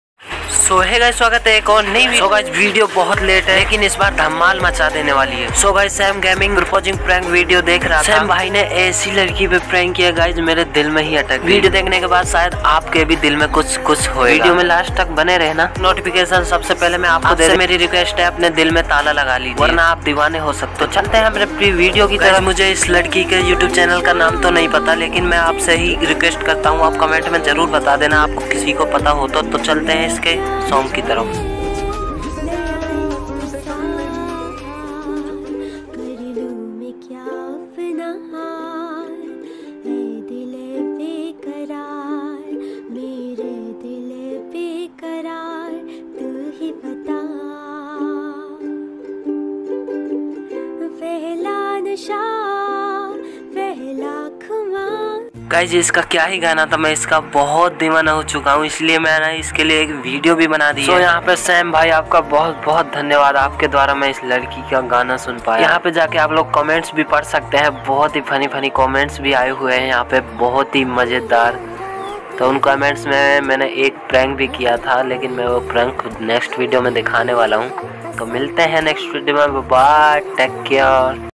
Payal gamig Is Love 😍 Beautiful Girl Streamer Proposing A Pillow | Prank With BKCG
Payal Gaming Is Love 😍Part-2 | Beautiful Girl Streamer Proposing A Pillow | Prank With BKCG

Credit:-
@Savage GIRL
@CASPERR GAMING
@Shreya Khirwal
@BKCG gaming
@PAYAL GAMING